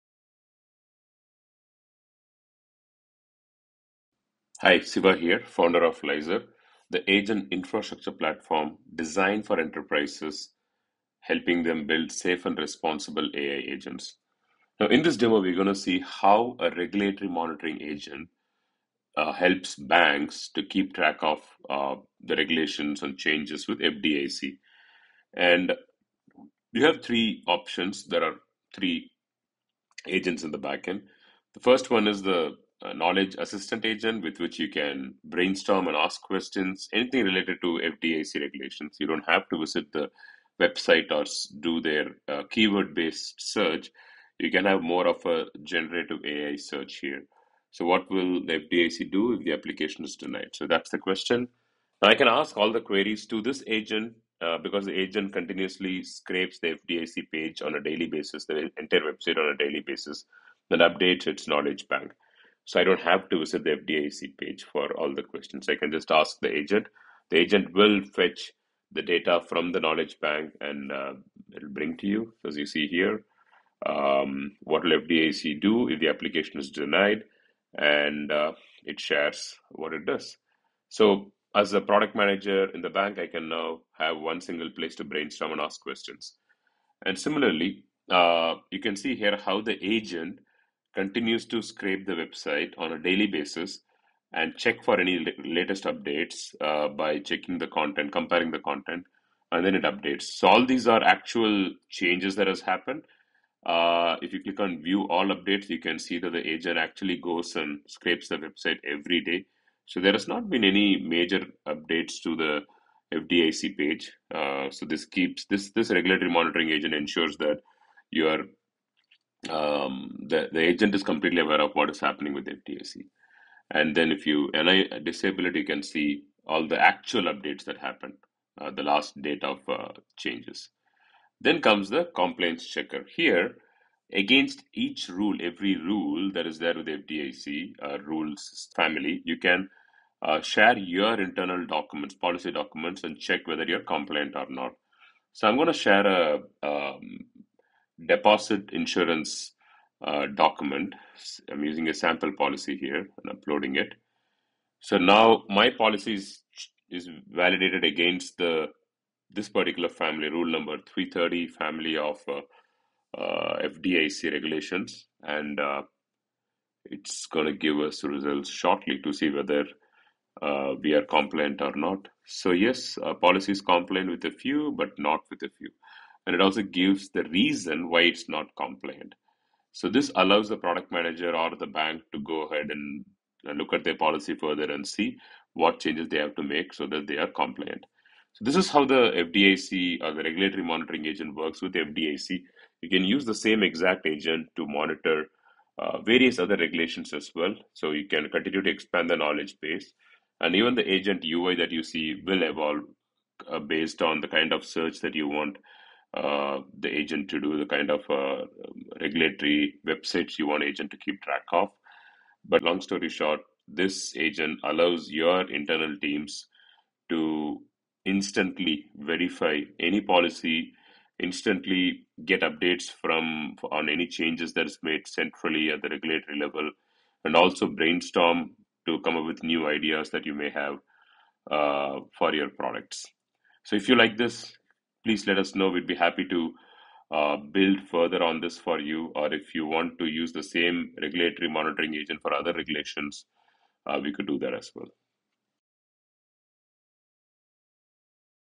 Automate Regulatory Compliance with Lyzr’s Regulatory Monitoring Agent
🏛️ Regulatory monitoring just became autonomous.

Every bank and financial institution faces one major challenge — staying up to date with evolving regulations like FDIC, FRB, or OCC. Manual monitoring is slow, error-prone, and expensive.

That’s why we built the Regulatory Monitoring Agent on Lyzr Agent Studio — an AI-powered compliance assistant that tracks, validates, and analyzes regulatory changes in real time.

💡 Here’s what it can do:

🧠 Daily FDIC monitoring: Automatically scrapes and updates regulatory content.

📚 Knowledge Assistant Agent: Lets you brainstorm and ask questions directly — no keyword search needed.

✅ Policy Compliance Checker: Upload internal policy docs to instantly verify FDIC compliance.

📈 Automated Alerts & Reports: Track regulatory updates and changes across multiple frameworks.

🌐 Expandable Framework: Extend to OCC, FRB, FINRA, or any global regulation.

Built for financial institutions, compliance teams, and product managers, this agent brings real-time clarity to a world of complex regulations.

📚 Chapters

00:00 – Introduction: AI Agents for Compliance
00:15 – The Regulatory Monitoring Agent Overview
00:30 – Knowledge Assistant: Ask FDIC Questions Instantly
00:50 – How the Agent Monitors FDIC Pages Daily
01:30 – Viewing and Tracking Regulatory Updates
02:10 – Policy Compliance Checker Demo
03:00 – How the Agent Validates FDIC Rule Families
03:45 – Real-Time Compliance Results
04:00 – Expanding to Other Regulations (FRB, OCC, FINRA)
04:30 – Final Thoughts and How to Get Started

🔗 Important Links